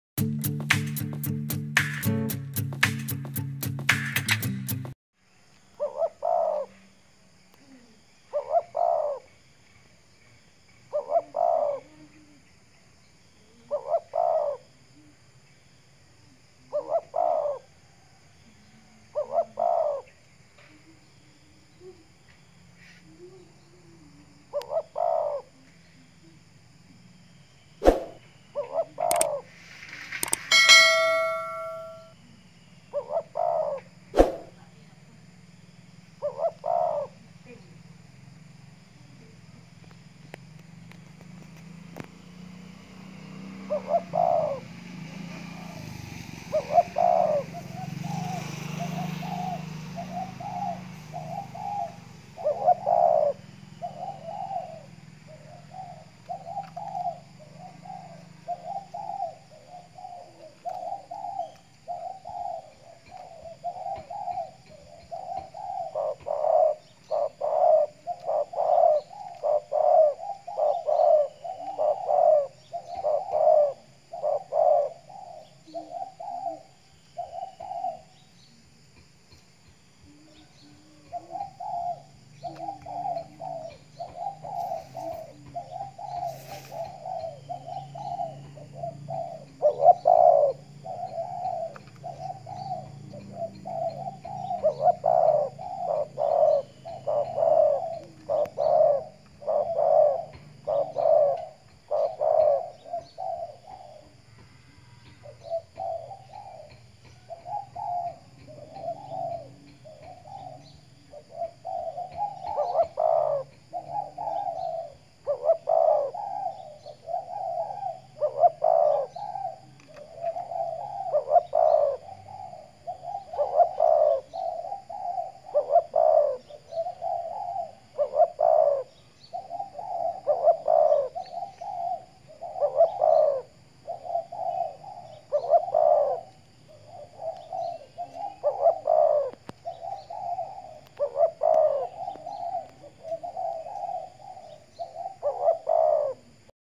tekukur pikat !! lagi di jemur biar fit~tekukur kepri
tekukur pikat lagi di jemur biar dia selalu sehat karna tekukur paling suka berjemur di pagi hari
semoga teman teman terhibur dengan video ini dan mohon dukungan nya teman teman...